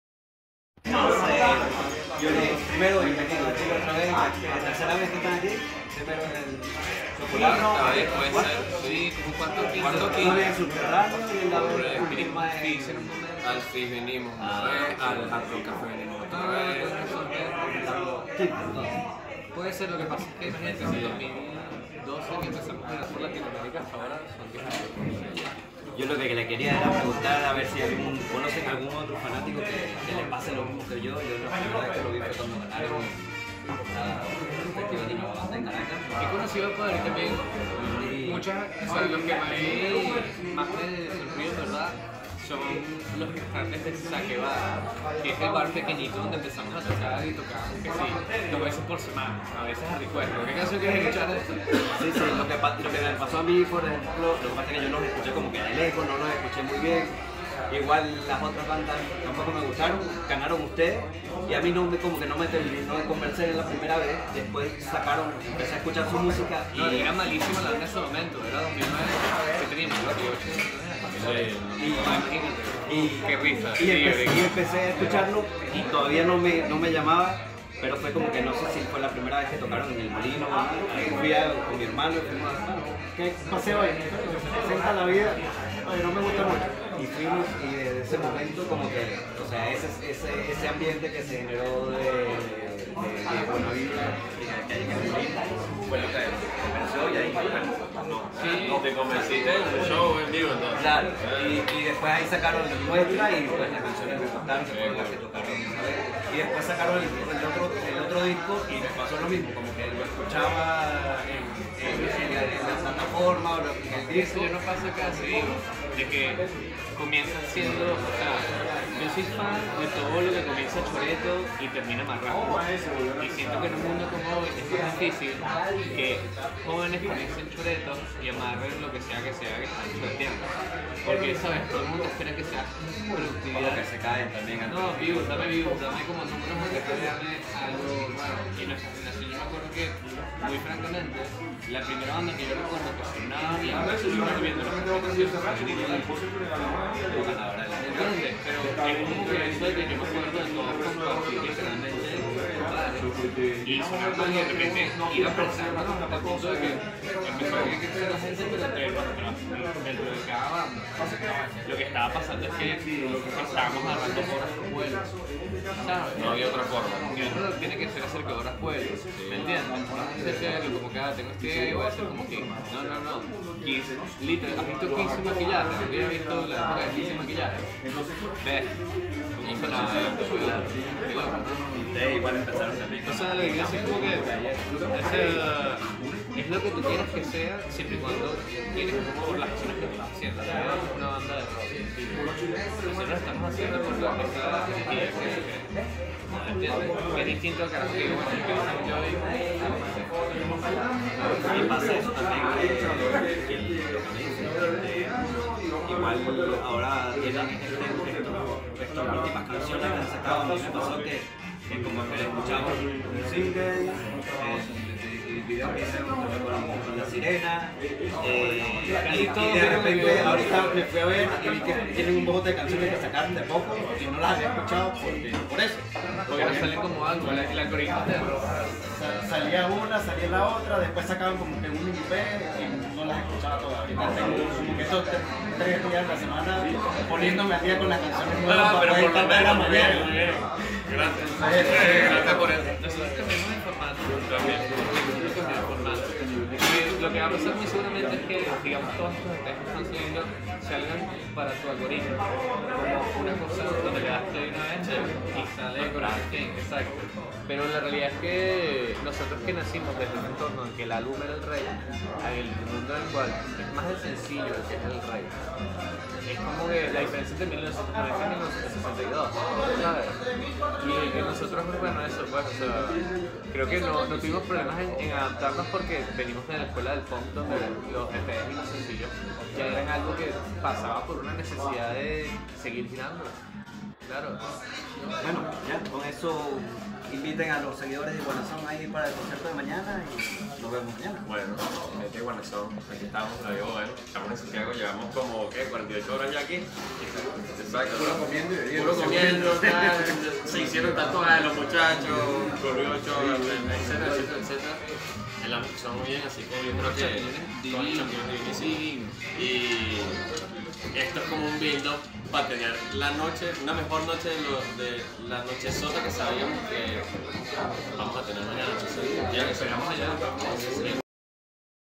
Charlando con la Vida Boheme - Santiago de Chile Octubre 2022
Aprovechando este espacio que ofreció la vida boheme para compartir con los medios y promocionar lo que seria su quinto concierto en Santiago de Chile